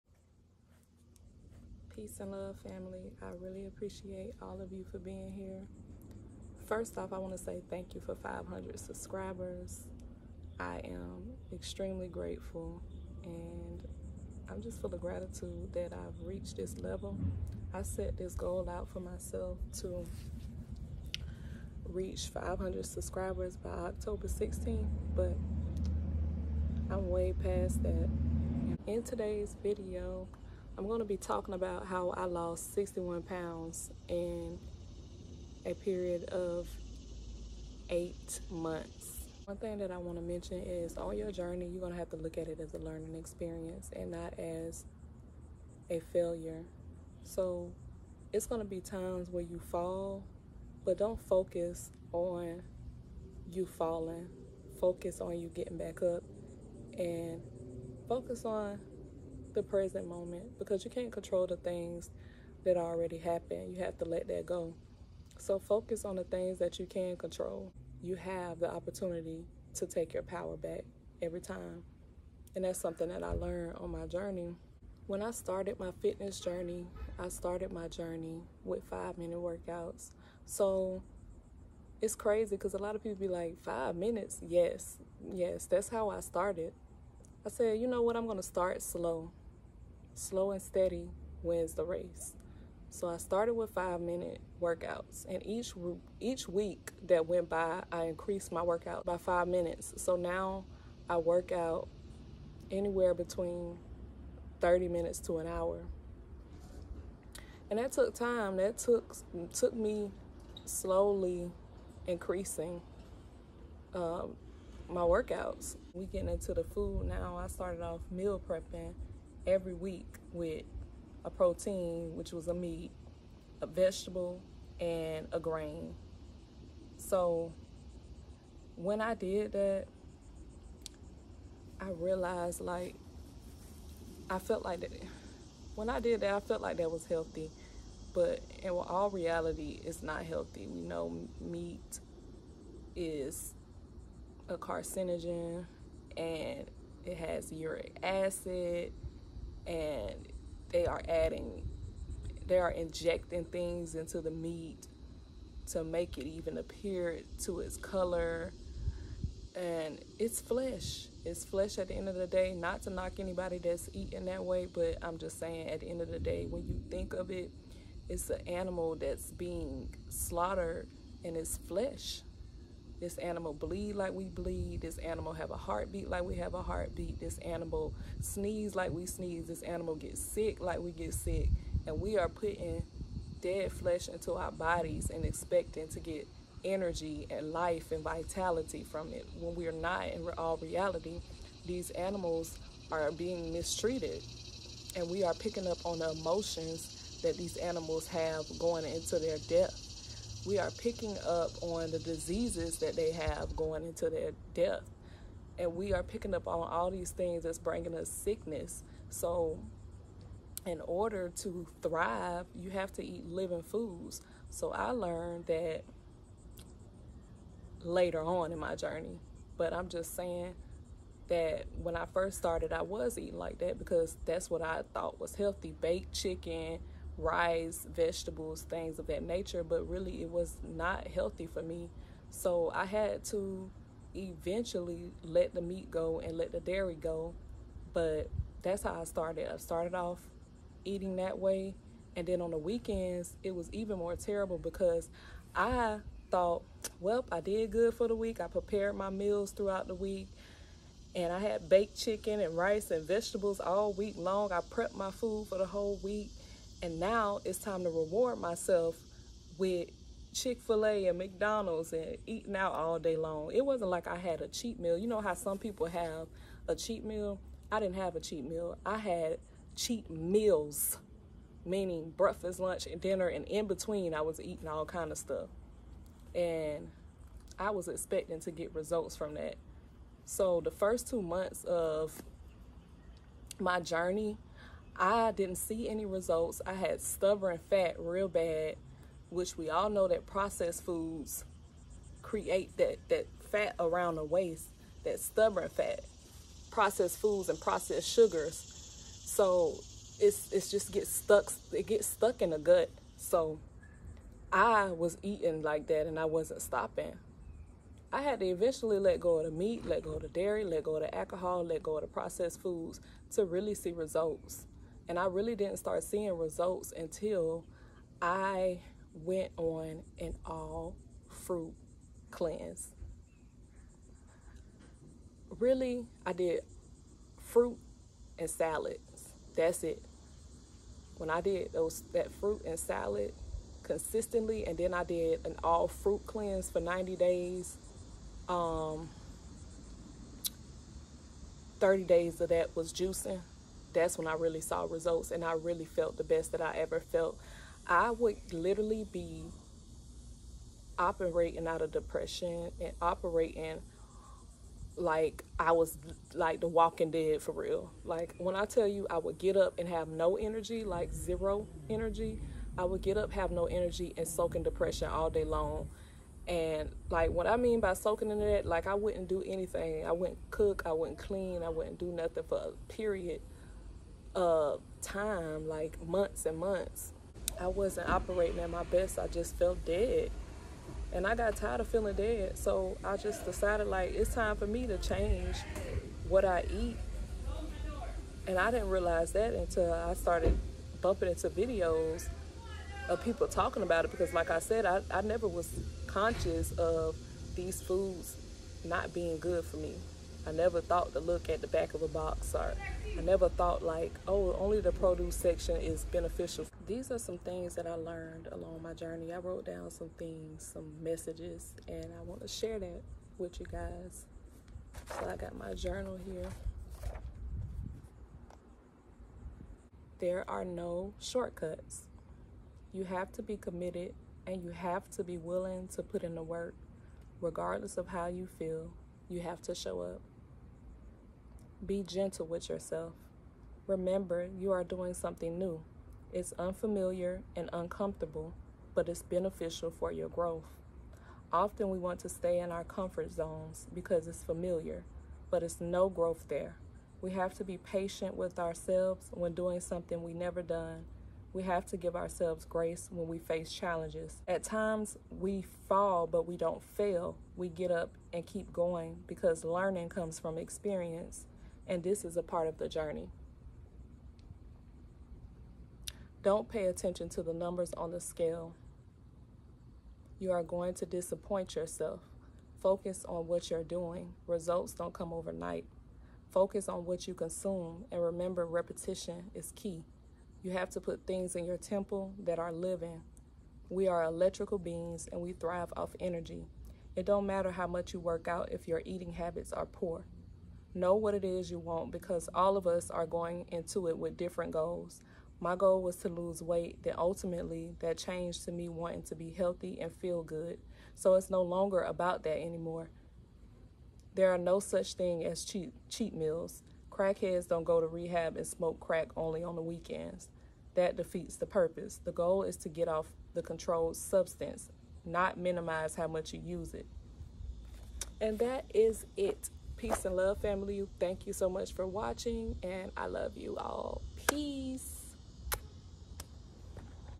How To Lose Weight Fast (I Lost 61 Pds Instantly By Doing This) 🤯
Greetings my beautiful tribe! In this video I’m sharing my weight loss journey. This is how I lost 61 Pds instantly. Lose weight fast by doing this..I love you all. Have a great day on purpose 💚